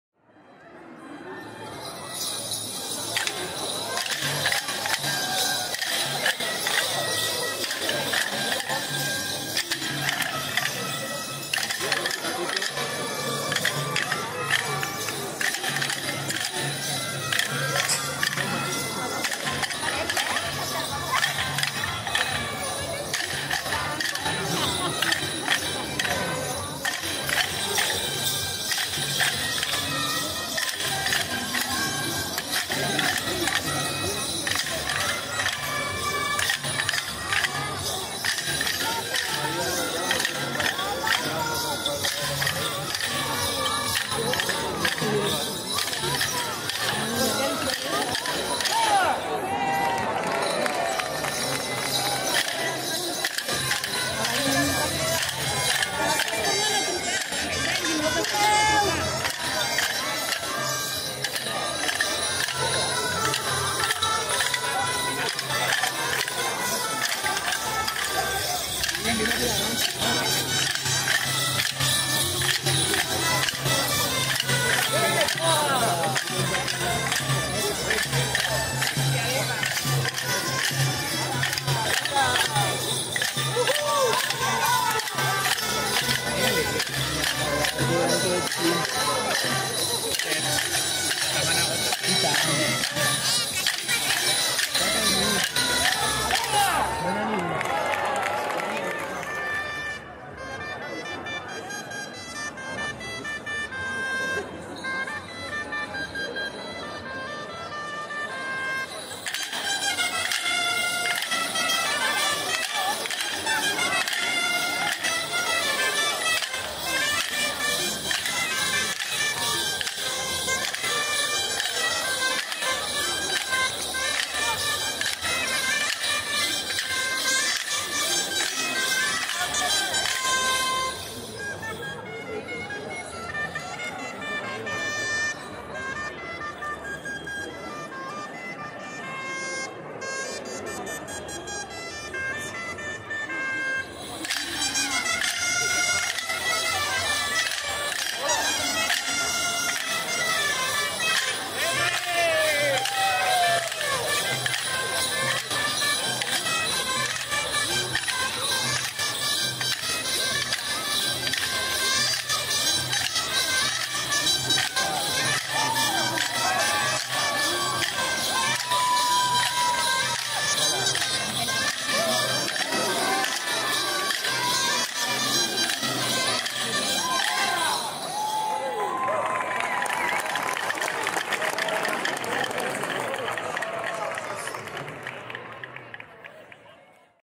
23/04/22 - Els Bastoners en Sant Jordi - Terrassa
Plaça Vella
41è Aniversari i Diada dels Bastoners de Terrassa